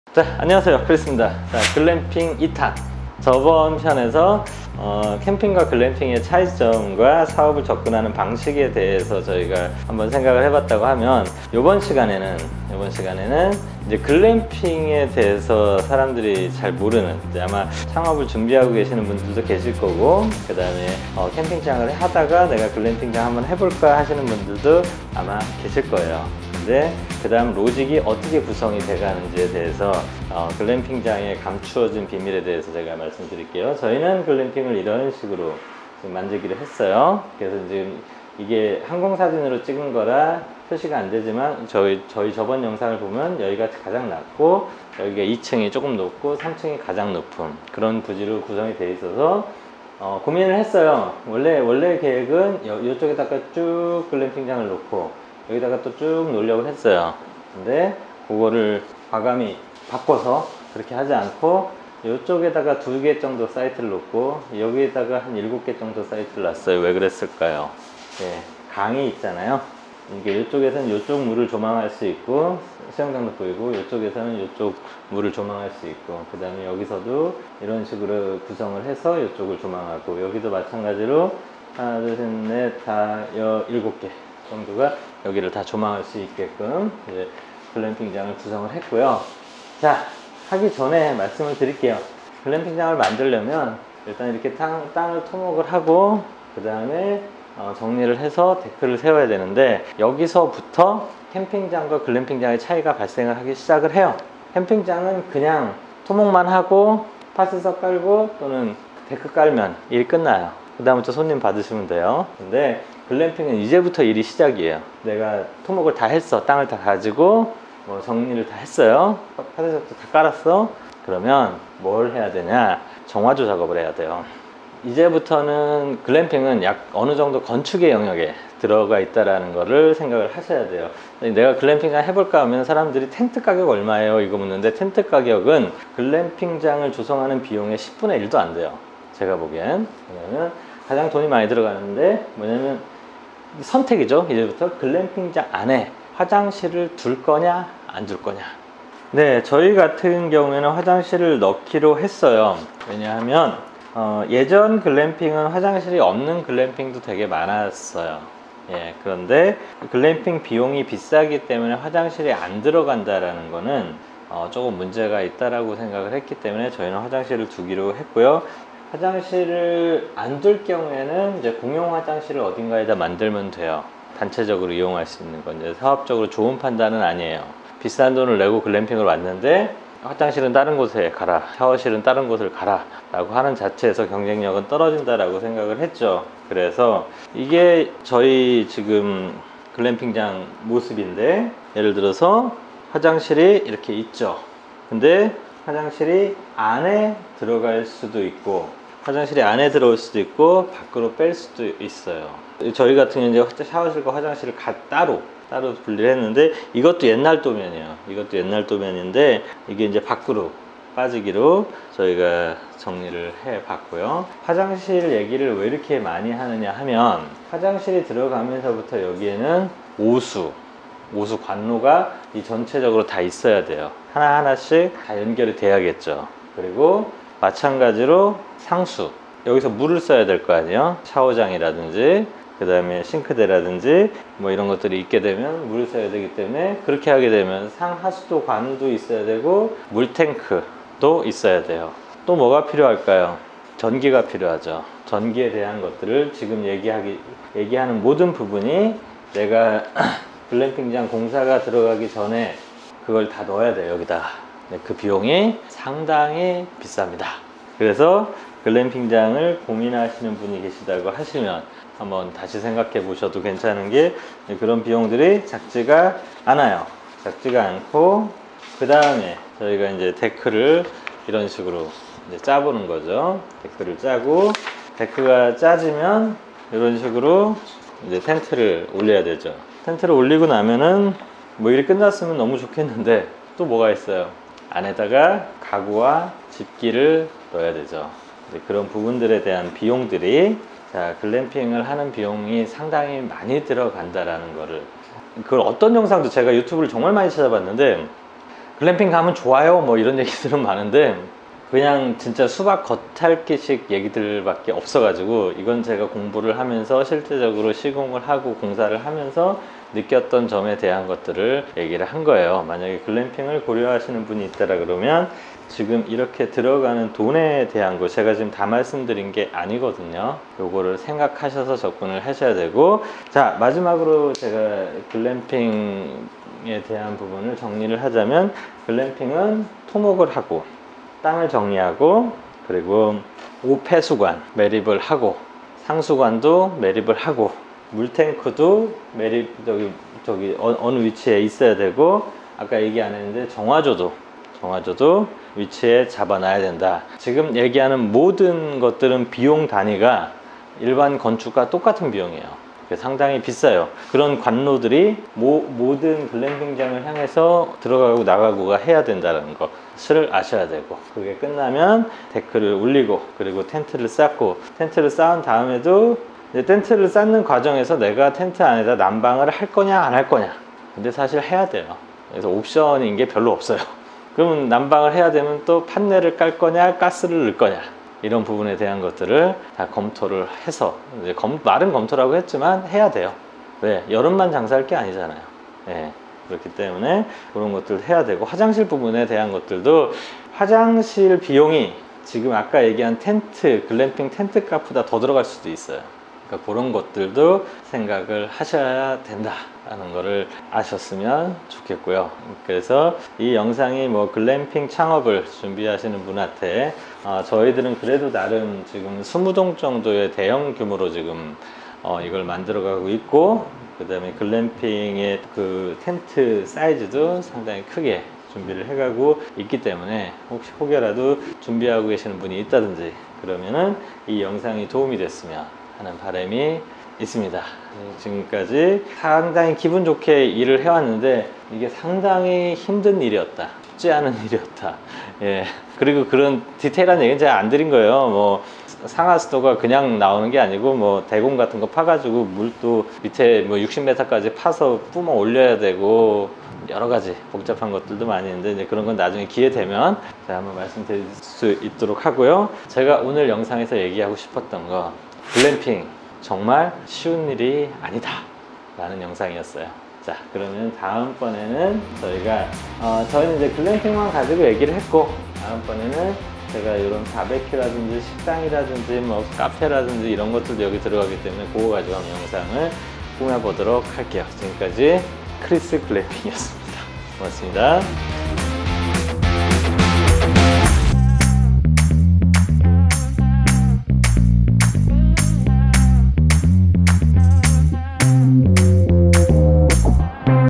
글램핑 20동... 텐트만 사면 될 줄 알았는데 큰 착각이었습니다
크리스월드 더 레지던스

주소
경기도 가평군 가평읍 북한강변로 360-91
경기도 가평군 가평읍 금대리 49-4

영업시간
매일 07:00~23:00